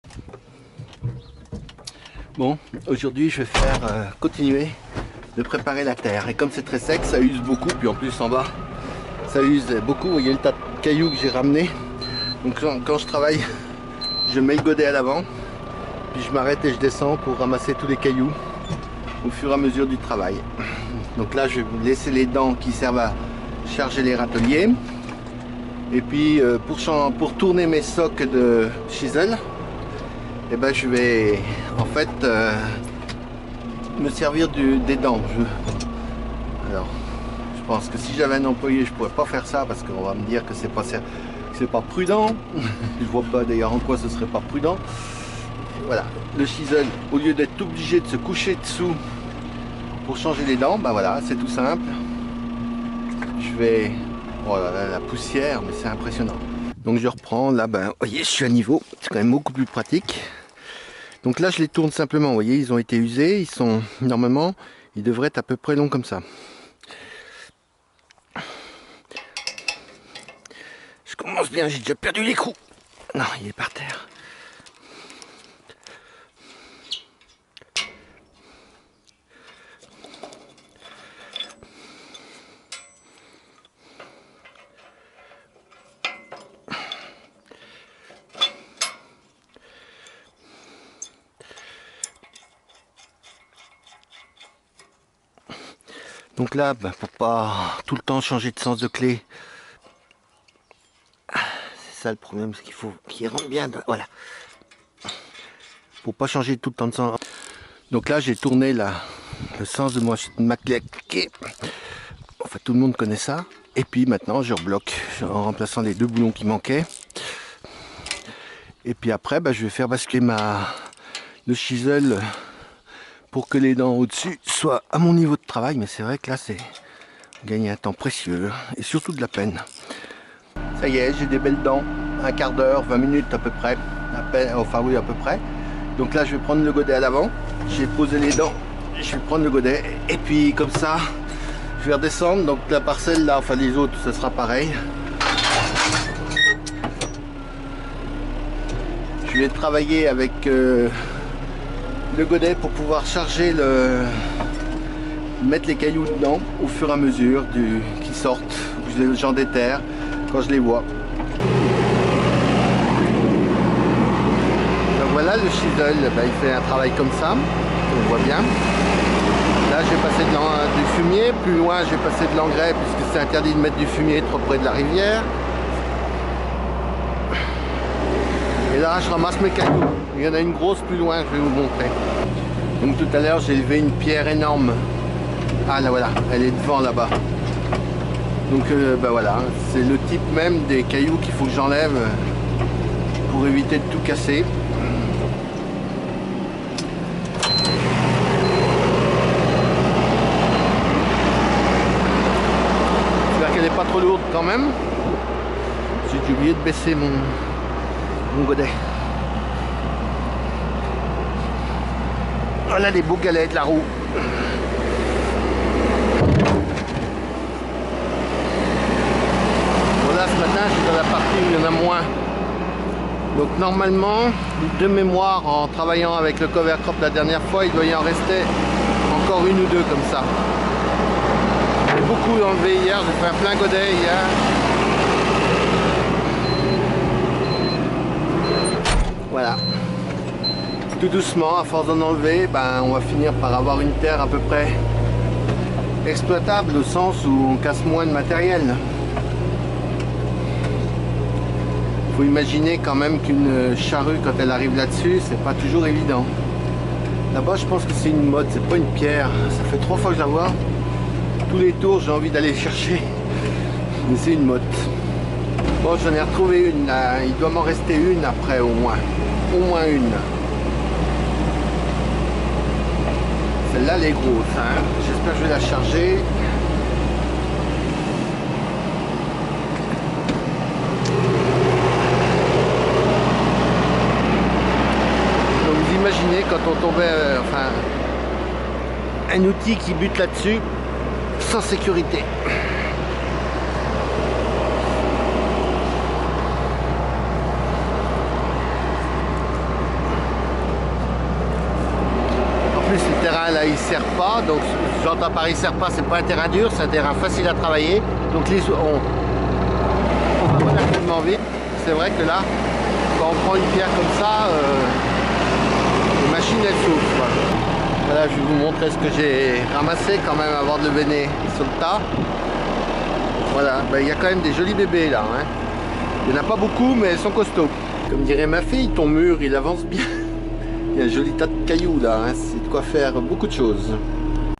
Préparation du semis simplifié avec le chisel
Avec la sécheresse, il faut changer les habitudes. Donc, je ne laboure pas cette année, sauf les prés s'il pleut avant la saint Martin. Pour le moment, ce n'est pas possible. Donc je prépare la terre pour semer cette semaine les parcelles déjà en culture cette année. Le changement de socs coûte environ 180 €. Le jeu, en le retournant, va me permettre de travailler environ une quarantaine d'hectares. Enfin, j'ai commencé le tas de pierres en été 2017. L' objectif est de faire des murs décoratifs dans la cour à ma retraite...